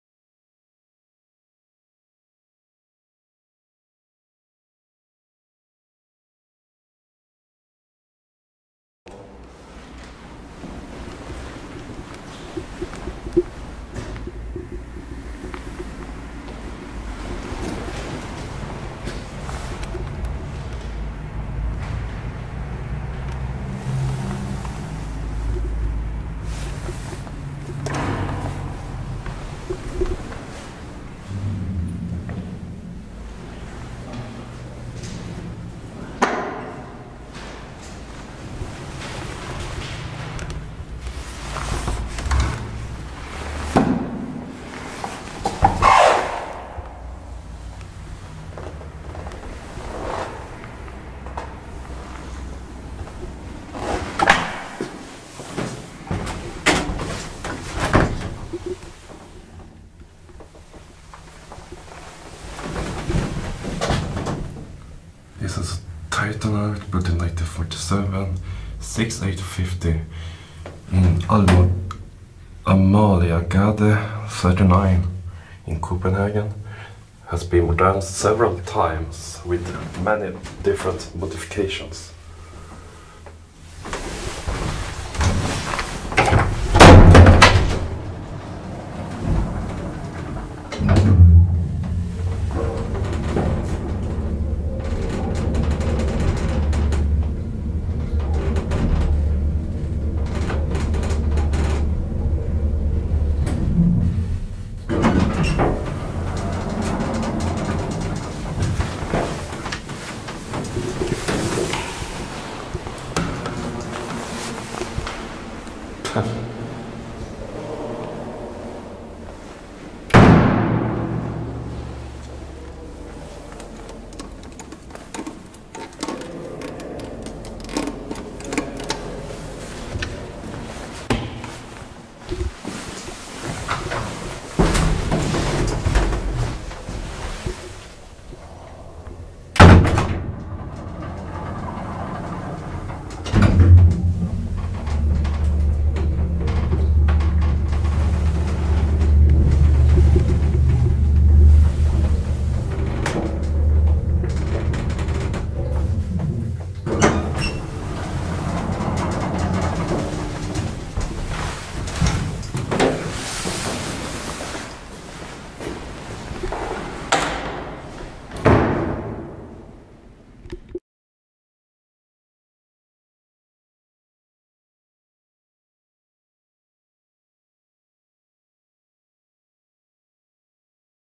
Epic Motor: 1947 Titan (MB. TT-KONE) Traction elevator @ Amaliegade 39, Copenhagen
Manufacturer: Titan
Installation year: 1947
Current moderniser: Trige-Titan-KONE
Latest modernisation year: 60's
NOTE: It got upgraded controllers since...
Serial Number:
Capacity: _Kg (_lbs.) - _ Persons
Floors: 6 = [K] S 1 2 3 4

Location: Amaliegade 39, Copenhagen DENMARK